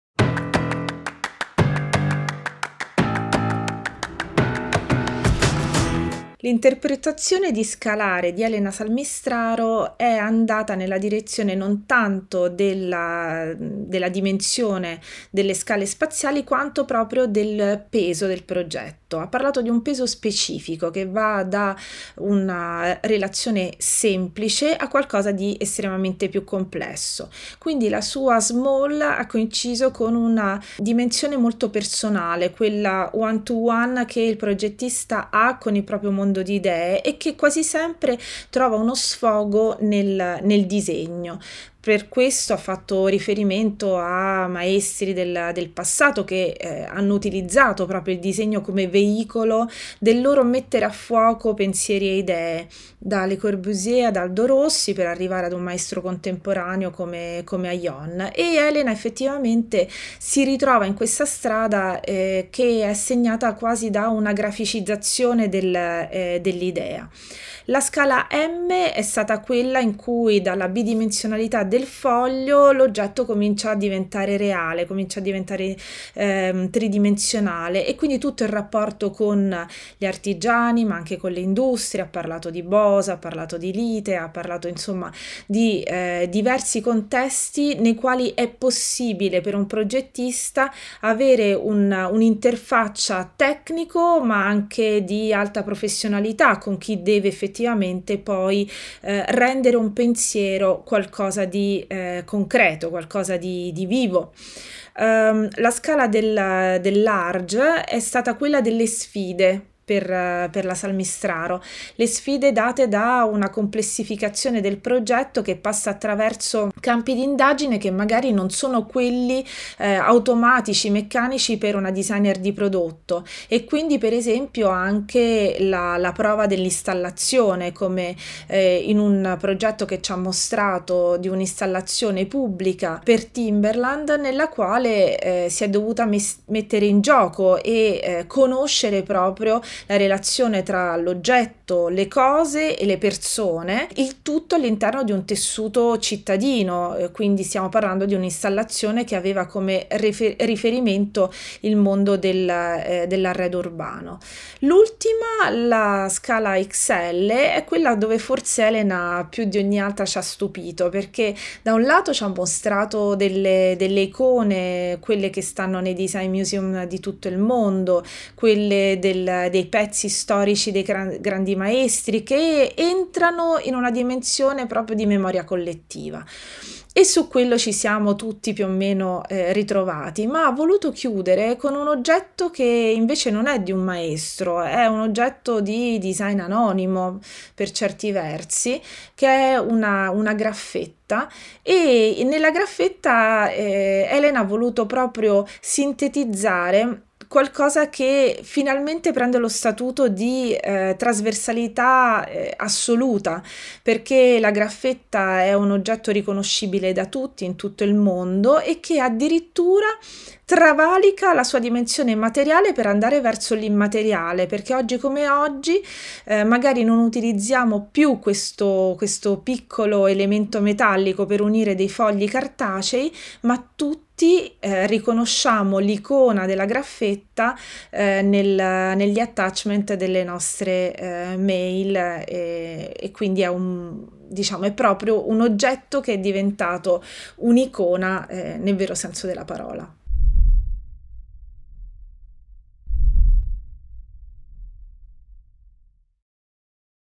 Scalare Reloaded - Elena Salmistraro
A great work of relationships and concreteness makes the “Scalare” version of Elena Salmistraro a complex and fascinating project. The inspiration comes from the masters of the past, who used drawing to define and focus their visions of art.